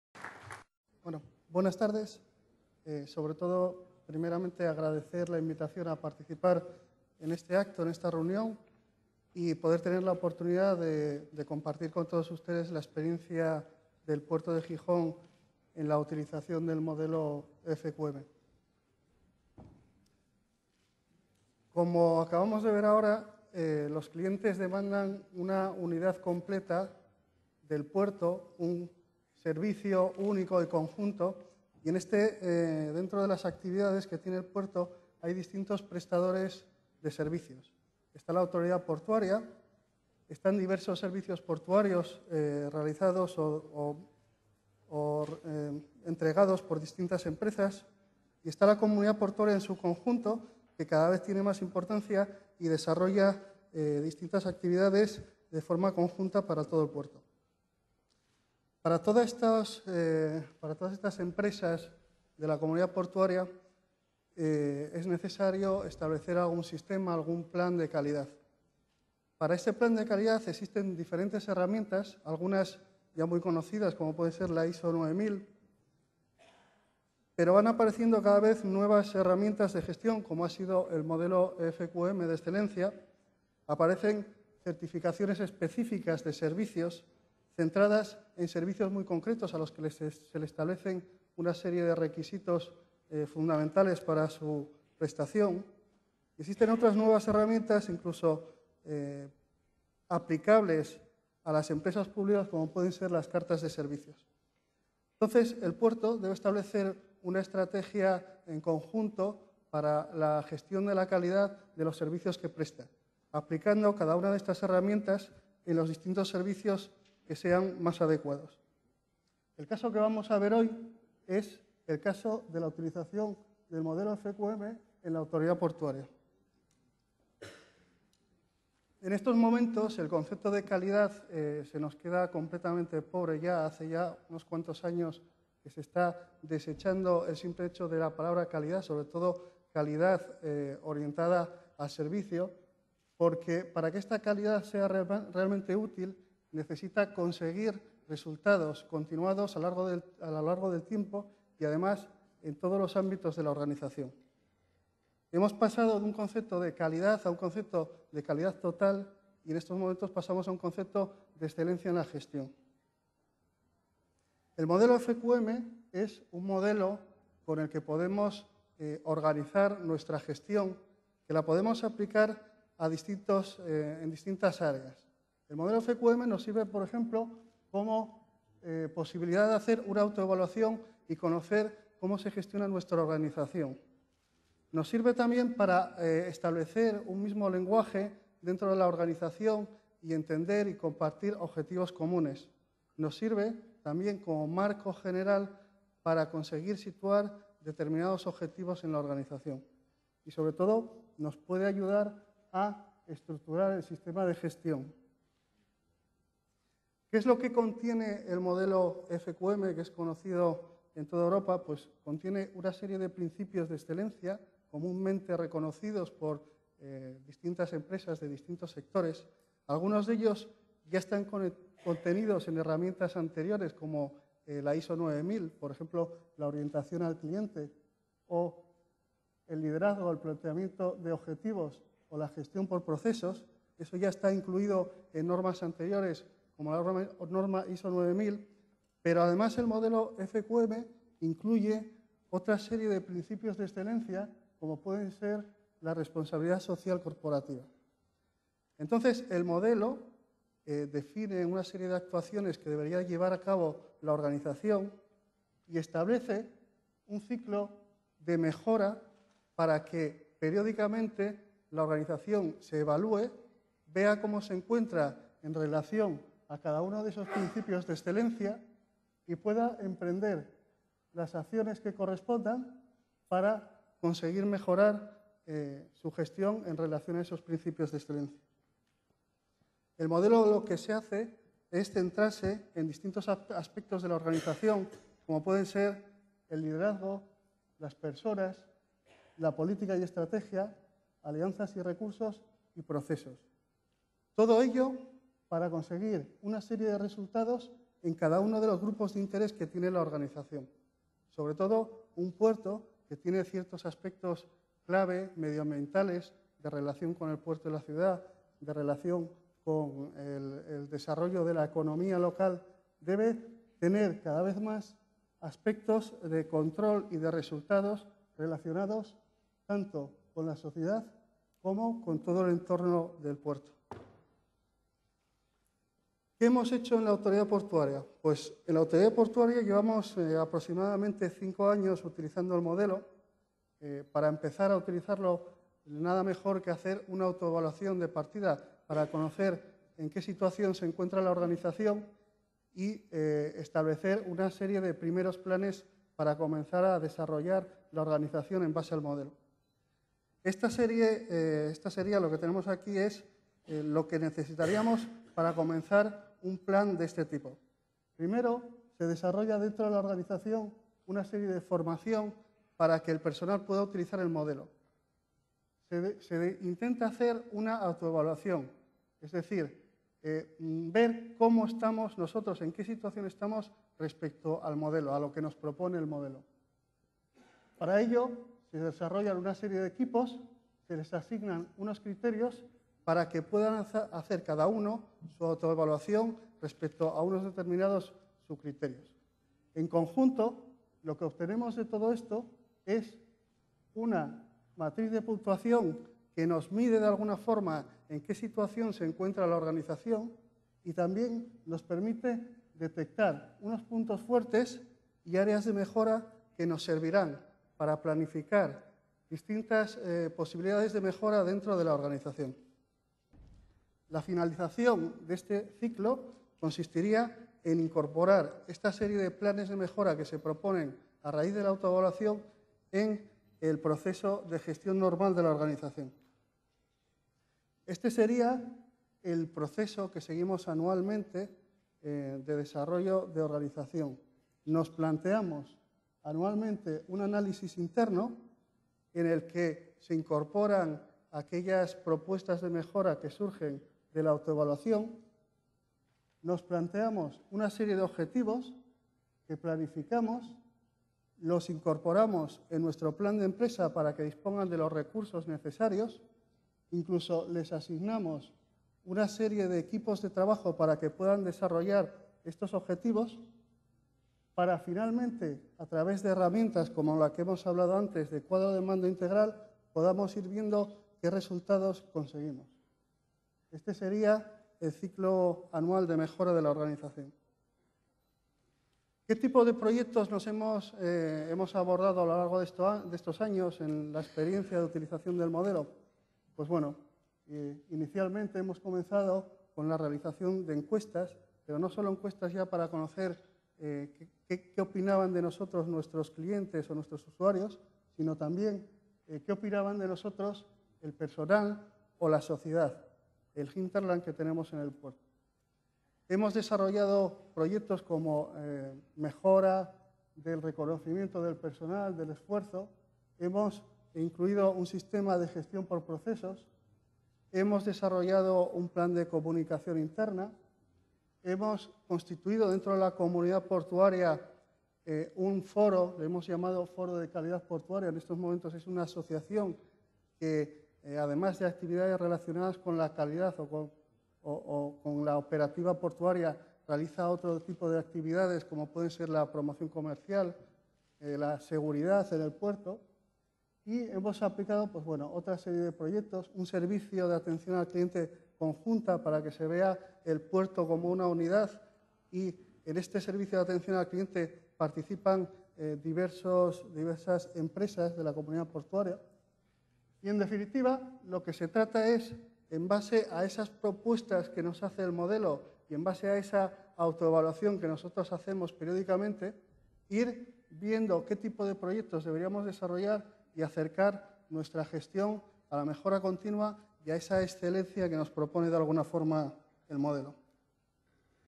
"Qualidade Global dos Portos" (XVI) -- J. C. Alvarez
Primeiro excerto (de um total de três) da intervenção de J. Carlos Alvarez (Departamento de Qualidade do Porto de Gijón), na conferência "Qualidade Global dos Portos", iniciativa que assinalou o início das comemorações do Bicentenário da abertura da Barra de Aveiro.
Auditório da Universidade de Aveiro, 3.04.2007